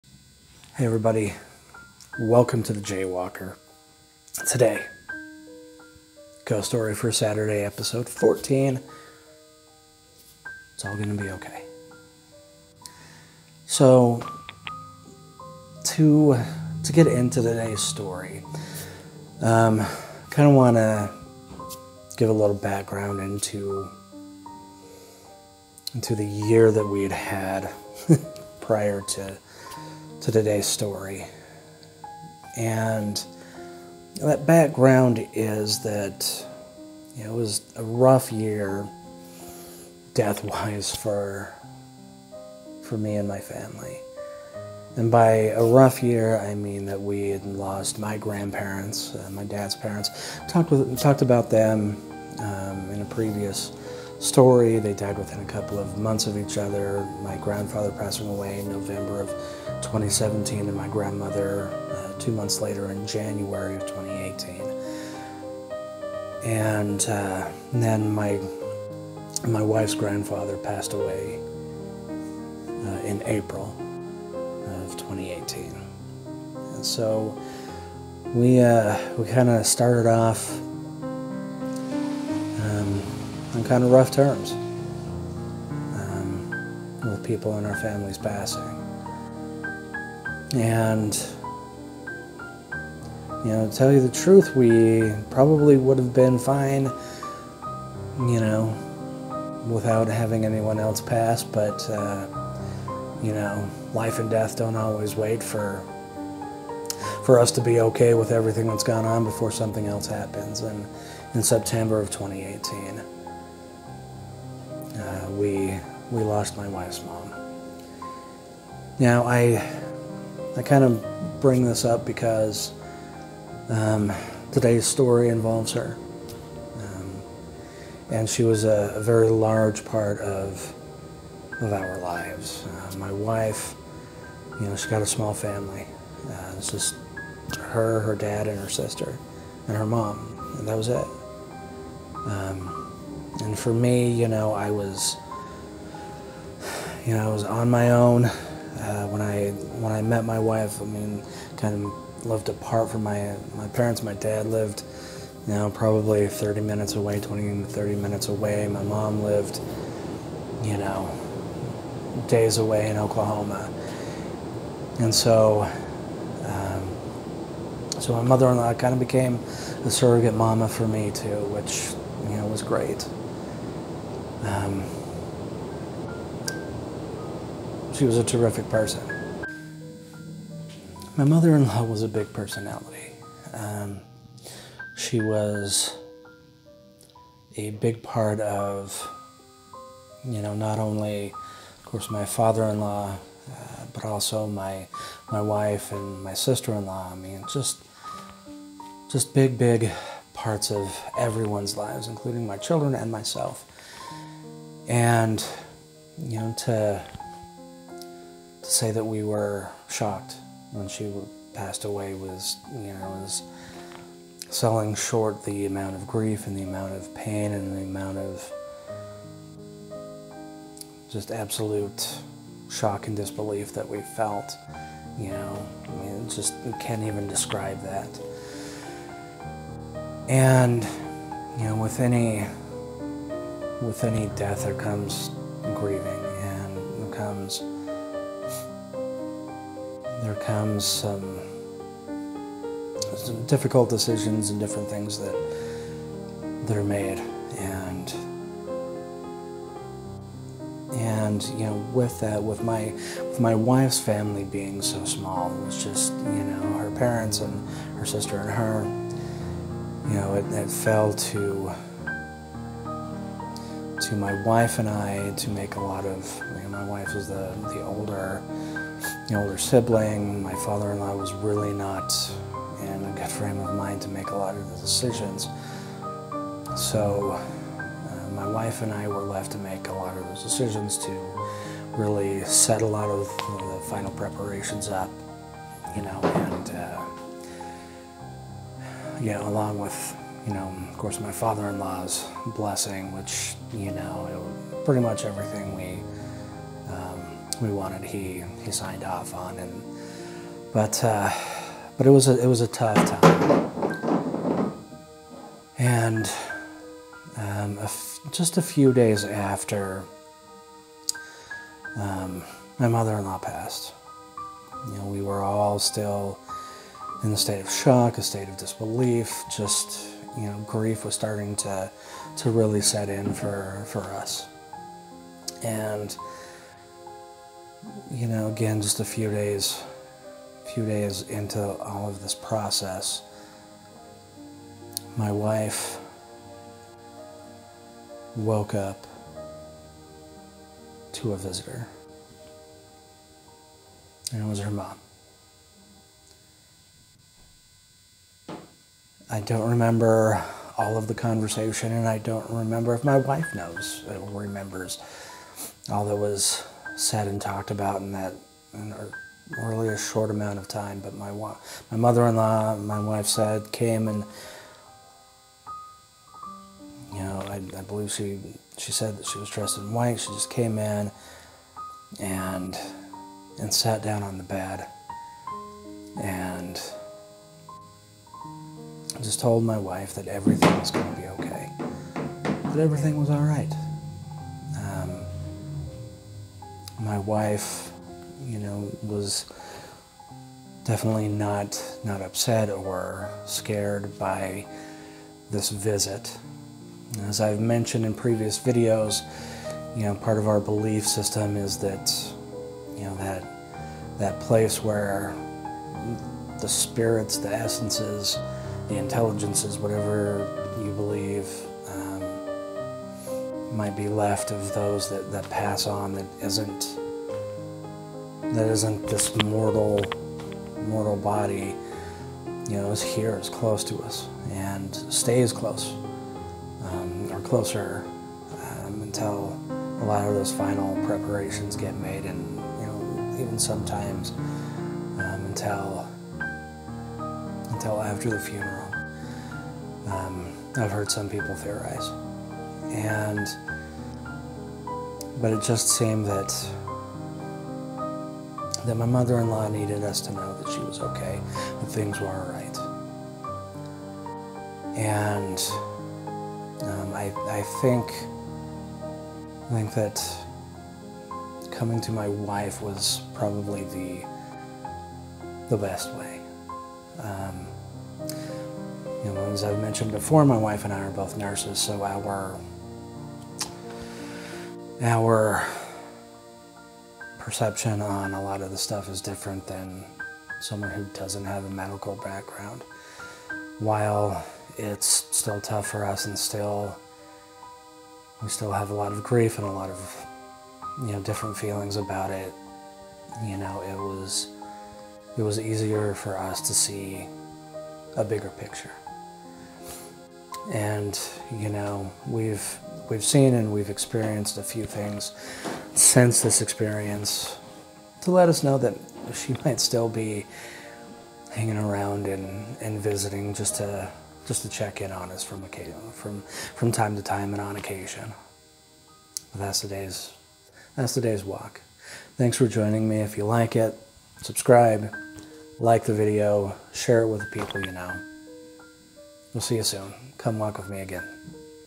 Ghost Story for a Saturday Episode 14: "It's gonna be ok"
A late night visit from a family member. Check it out.